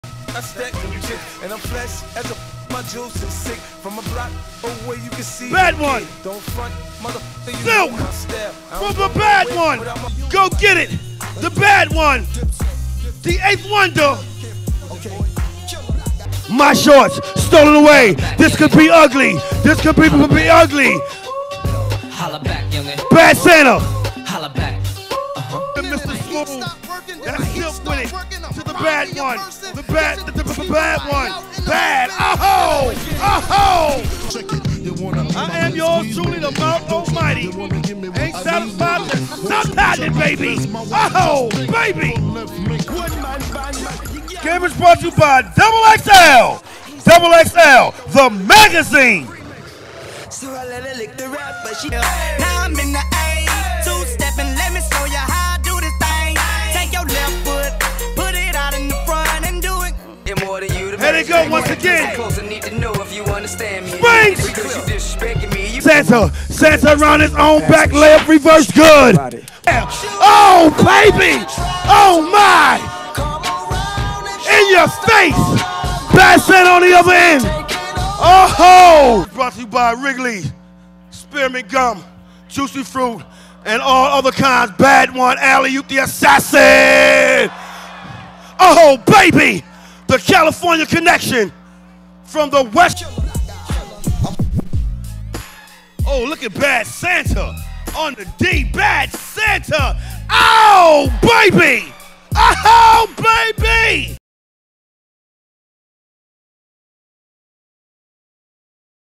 AND1 the Bad One Bad santa making everyone better
AND1 2007 original footage Bad One doin what he do..Making everyone better